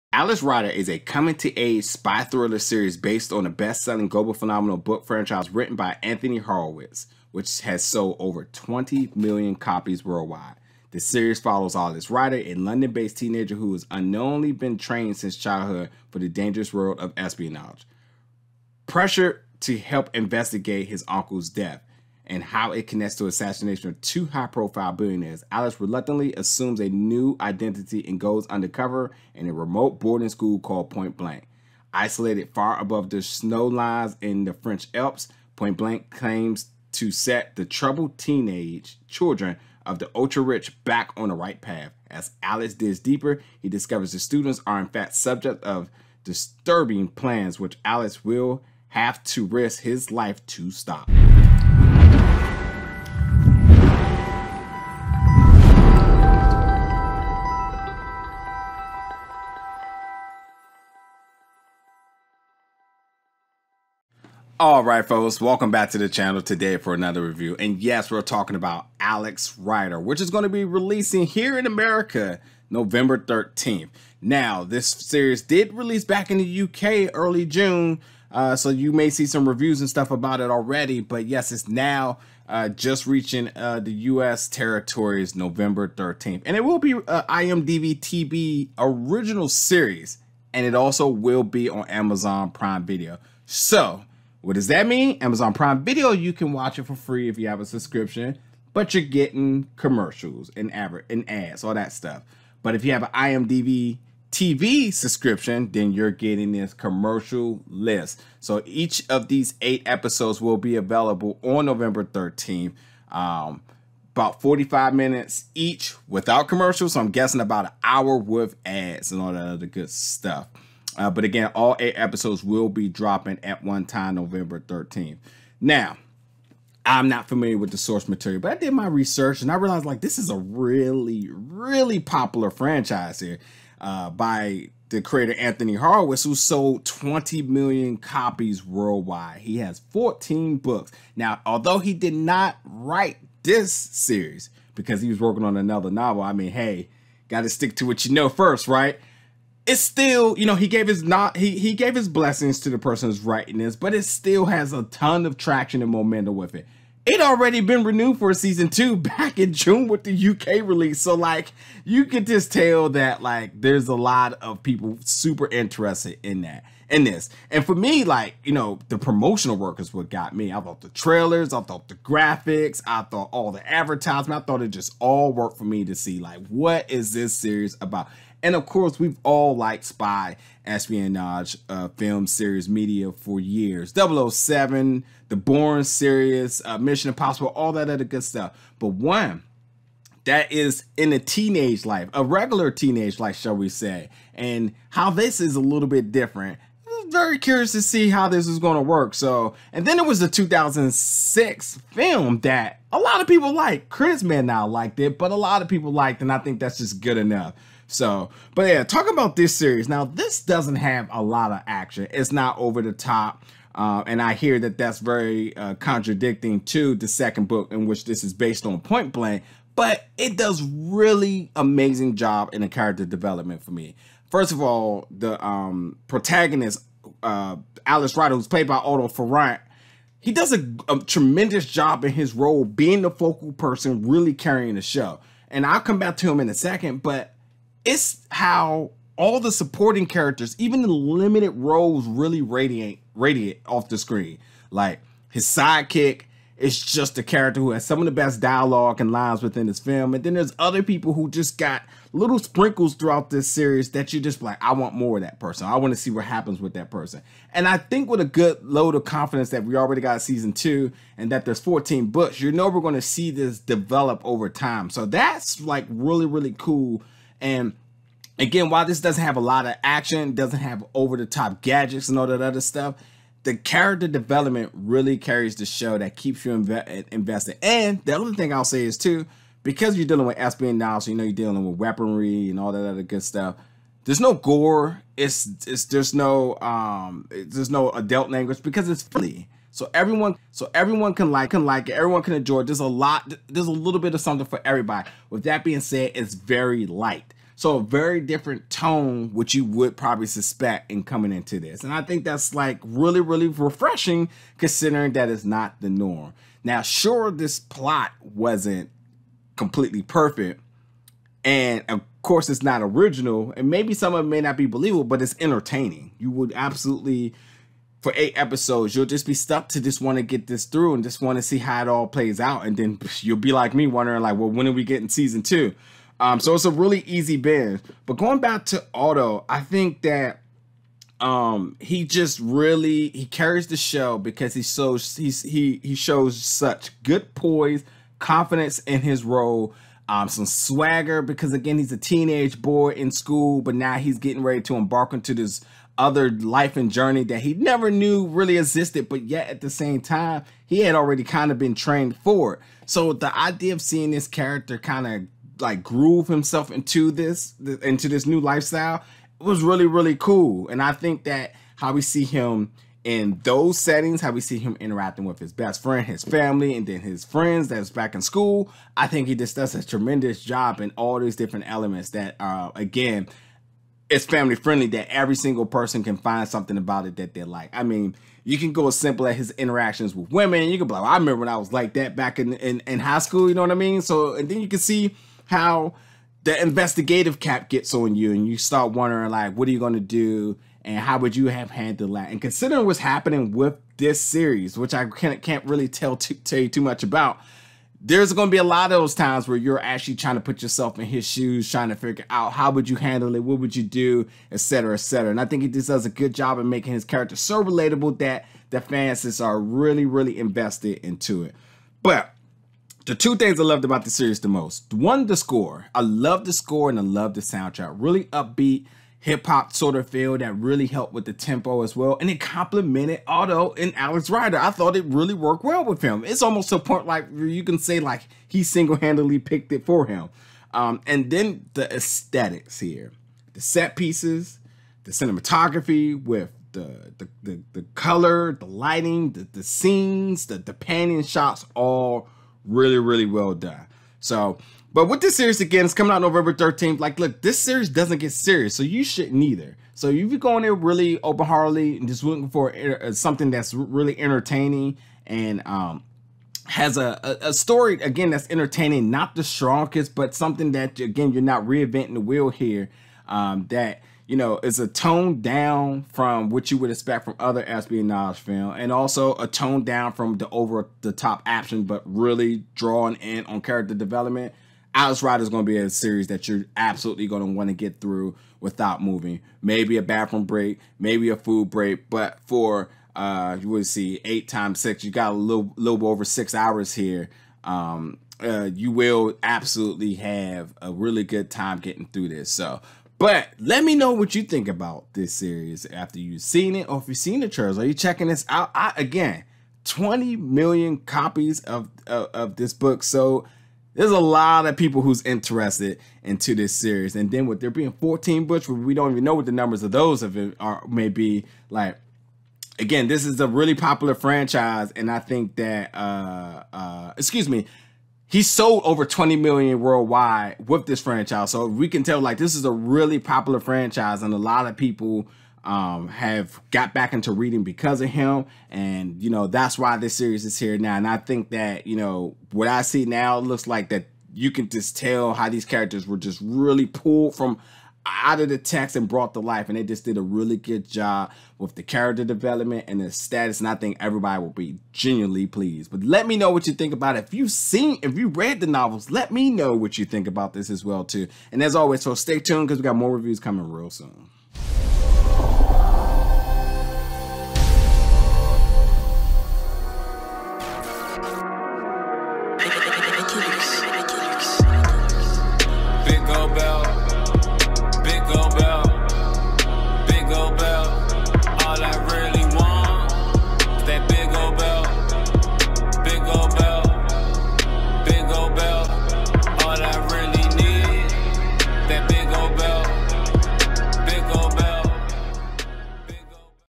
Alex Rider Review (2020) | Amazon Prime Video (IMDb TV ORIGINAL) SERIES REVIEW
Big Gold Belt Media Reviews IMDb TV ORIGINAL SERIES ALEX RIDER starring  Otto Farrant, Ronke Adekoluejo, Brenock O'Connor & Creator Guy Burt.

Alex Rider is a coming of age spy-thriller series based on the best-selling, global phenomenon book franchise written by Anthony Horowitz, which has sold over 20 million copies worldwide.  The series follows Alex Rider, a London based teenager who has unknowingly been trained since childhood for the dangerous world of espionage.  Pressured to help investigate his uncle’s death, and how it connects to the assassination of two high-profile billionaires, Alex reluctantly assumes a new identity and goes undercover in a remote boarding school called Point Blanc.  Isolated far above the snowline in the French Alps, Point Blanc claims to set the troubled teenage children of the ultra-rich back onto the right track.  As Alex digs deeper, he discovers the students are in fact the subjects of a disturbing plan which Alex will have to risk his life to stop.  The season is based on the second book in the franchise, Point Blank.

* Be sure to visit us at our for additional news and updates